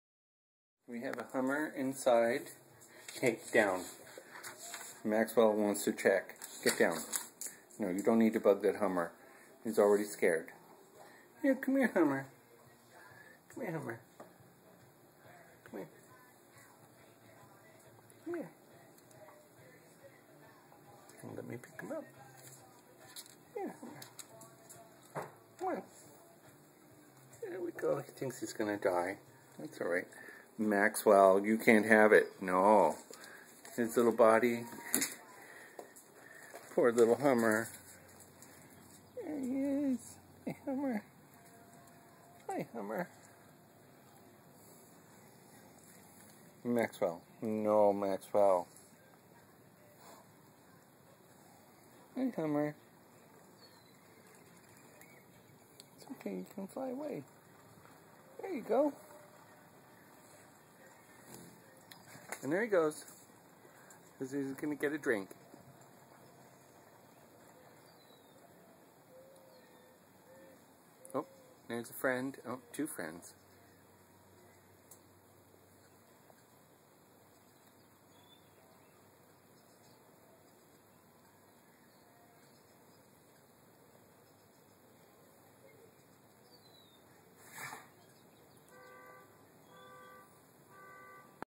Hummingbird Inside!
A hummingbird gets stuck inside and I save it.